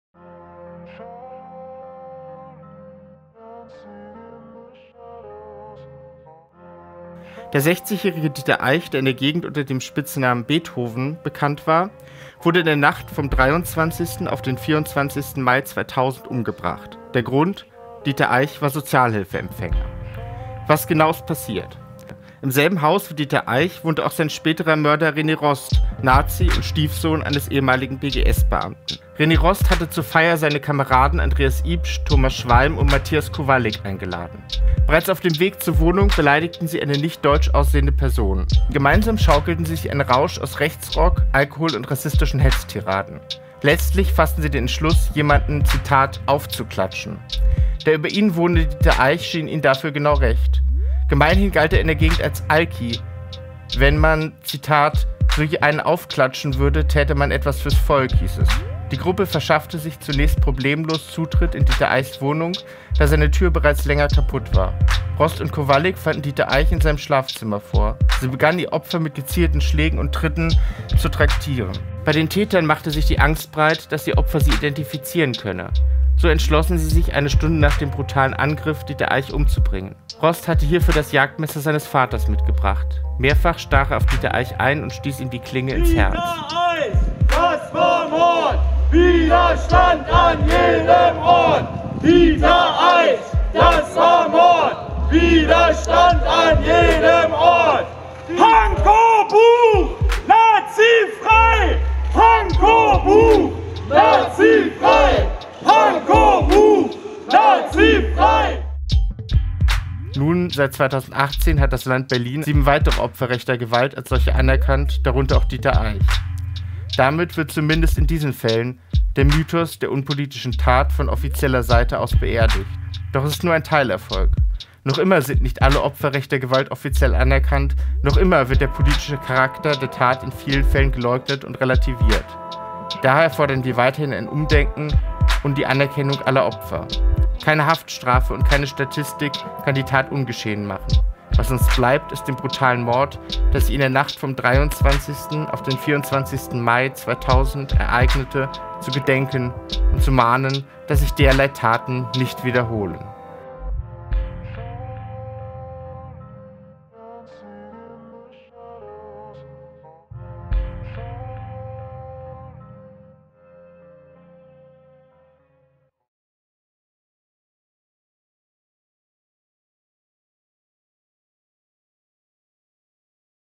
In Gedenken an Dieter Eich
Kommt zur Einweihung der Gedenktafel für Dieter Eich!
Niemand ist vergessen!

Di. 24. Mai 2022 | Berlin-Buch
17:00 Uhr | Walter-Friedrich-Str 52

Anreise:
Gesundbrunnen: Abfahrt 16:16 Uhr
S-Bhf. Pankow: Abfahrt 16:20 Uhr
(letzter Waggon)

Mehr Infos unter: berlin.niemandistvergessen.net